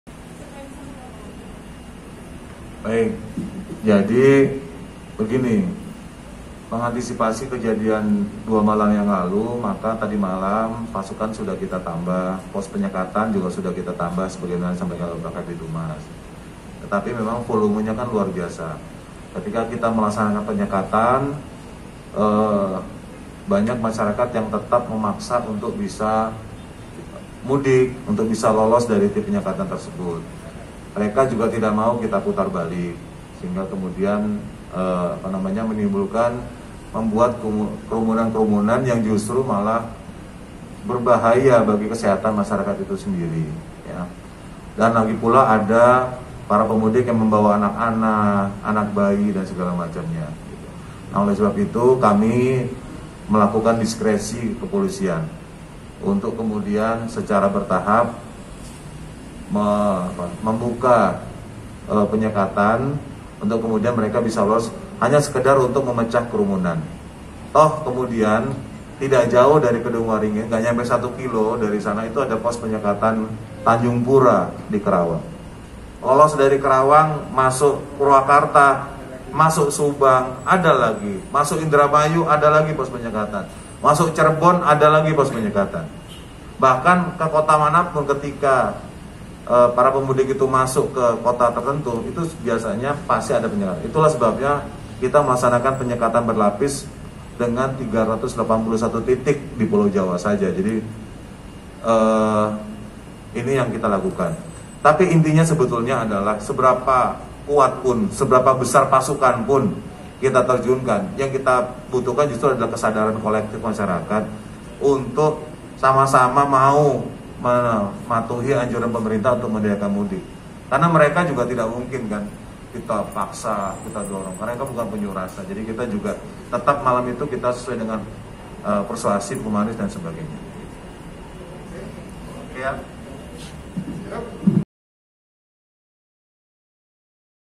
Demi Memecah Kerumunan, Polda Metro Loloskan Pemudik Sepeda Motor di Kedungwaringin
Direktur Lalu Lintas Polda Metro Jaya Kombes Sambodo Purnomo Yogo mengatakan untuk mengantisipasi jebolnya pos penyekatan pemudik di Kedungaringin, Bekasi, pihaknya sudah menambah jumlah pasukan dan menambah pos penyekatan di sana sejak Senin (10/5/2021) malam.

Tapi katanya volume kendaraan terutama sepeda motor yang hendak mudik keluar Bekasi menuju arah ke Karawang cukup besar dan luar biasa.